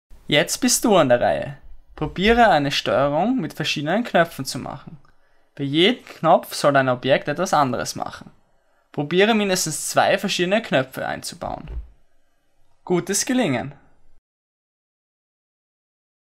[PCMOOC] 2.4 Aufgabe: Eine kleine Button Steuerung
Schaue dir das Video an und probiere die Aufgabe zu lösen. Programmiere eine Steurung . Dazu müssen die verschiedenen Objekte miteinander kommunizieren.